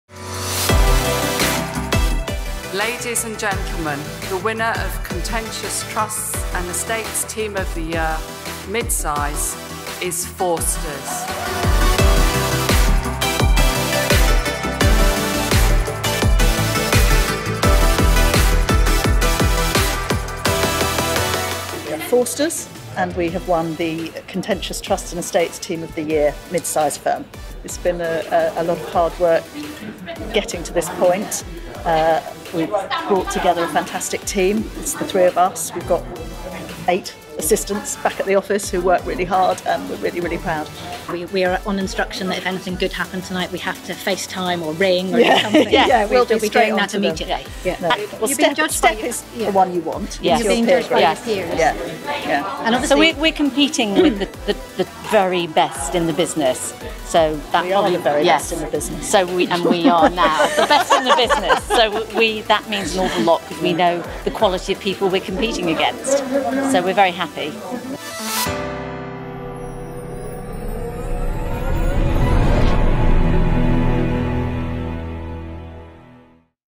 STEP PCA 2019: Contentious Trusts and Estates Team (Midsize)
Forsters receives the 2019 STEP Private Client Award for Contentious Trusts and Estates Team of the Year (Midsize)